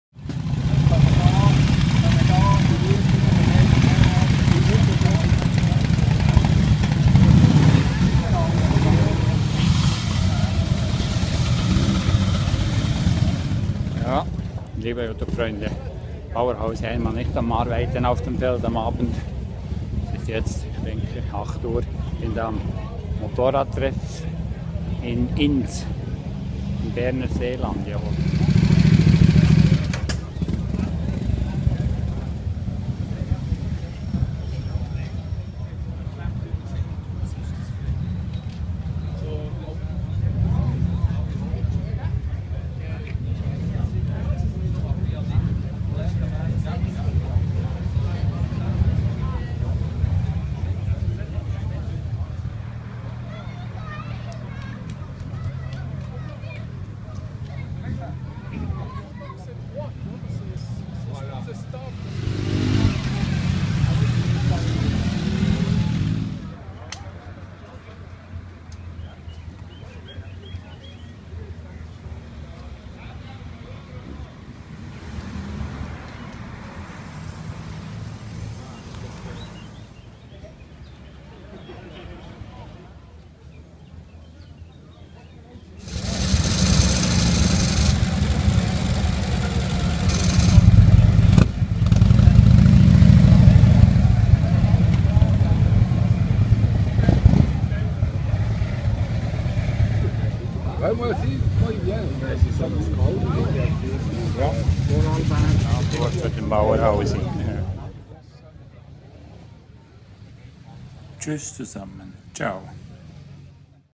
Motorradtreffen Ins CH
Ins im Berner Seeland, zwischen Bieler, Neuenburger und Murtensee
jeden Mittwochabend